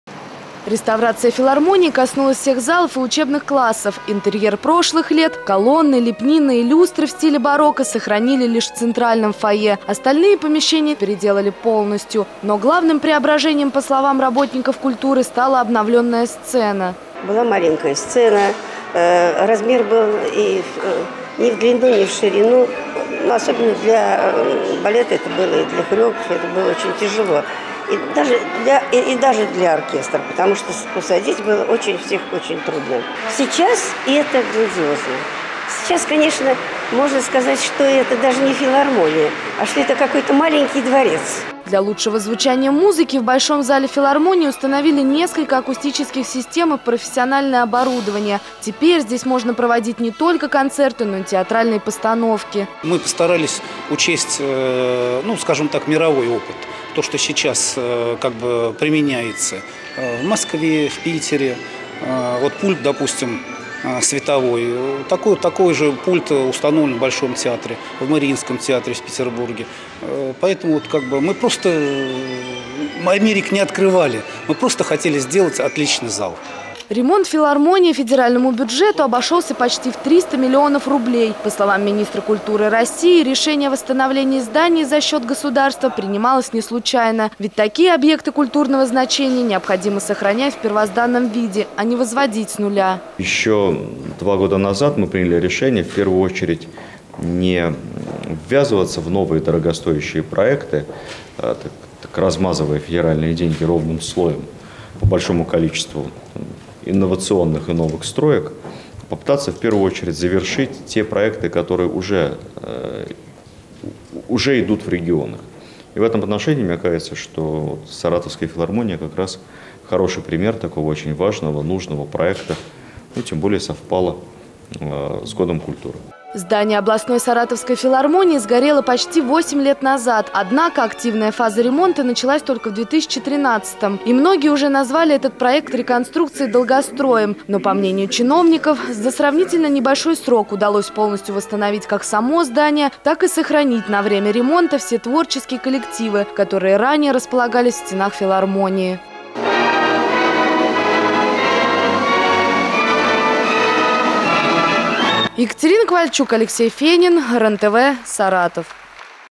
МЕДИНСКИЙ В ФИЛАРМОНИИ
В минувшую пятницу с рабочим визитом в  Саратов прибыл министр культуры России. Владимир Мединский вместе с губернатором нашего региона официально открыли здание отремонтированной областной филармонии. Что удалось сохранить в первозданном виде, а что перестроили полностью после пожара, узнала Екатерина Ковальчук. Сюжет вышел в эфир 19 мая.